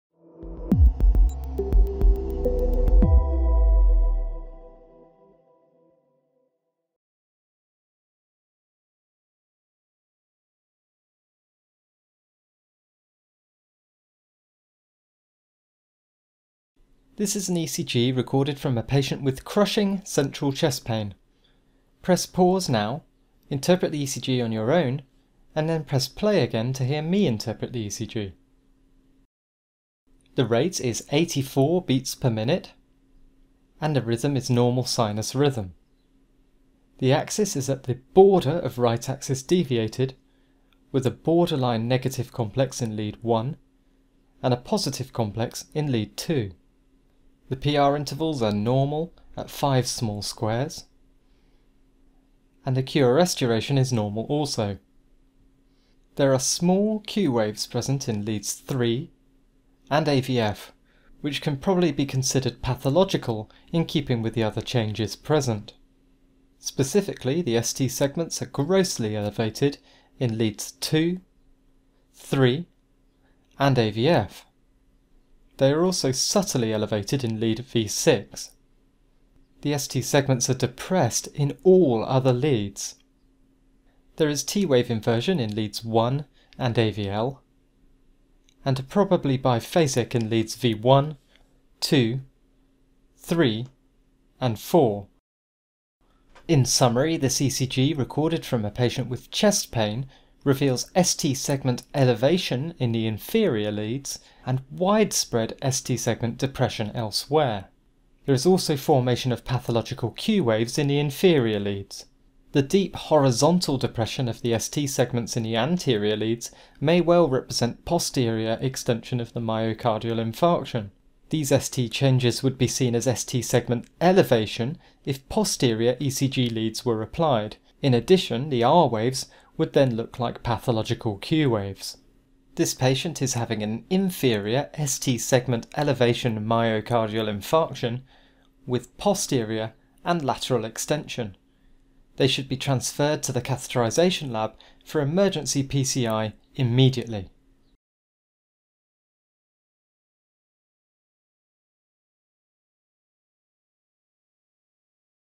ECG Case 17 - Crushing Chest Pain
A 39 year old patient presents to the emergency department with crushing central chest pain. He has diabetes and is obese. This is his ECG recorded in the presence of chest pain. What is your interpretation?